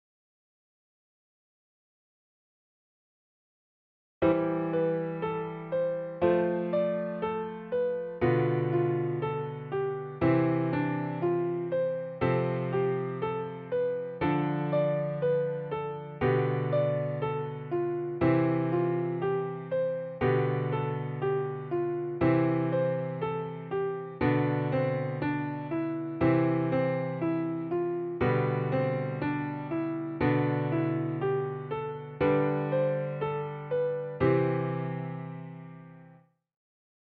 Fractal Music Sample 1 - Iterated Fractal Method
Sample of fractal music composed using Iterated Fractal Systems Method.